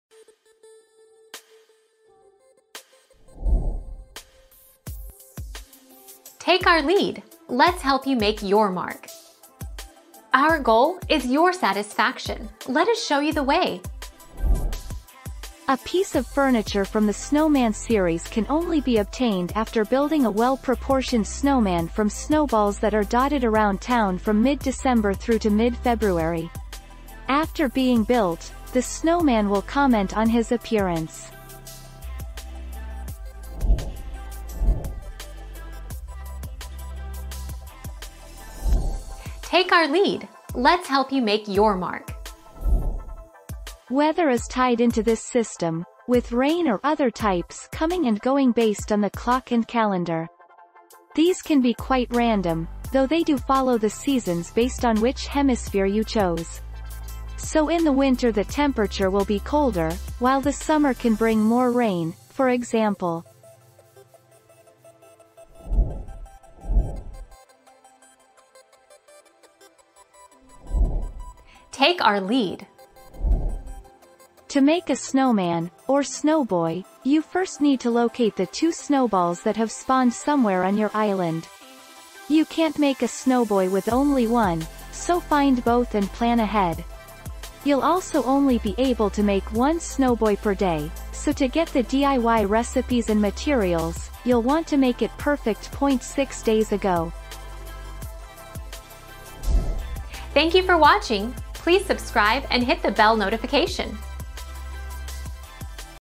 How do you get snowman furniture in Animal Crossing?
00:00 - How do you get snowman furniture in Animal Crossing?
00:35 - How does Animal Crossing determine weather?
01:07 - How many snowboy can you make a day?

Laura S. Harris (2021, January 18.) How do you get snowman furniture in Animal Crossing?
    AskAbout.video/articles/How-do-you-get-snowman-furniture-in-Animal-Crossing-212348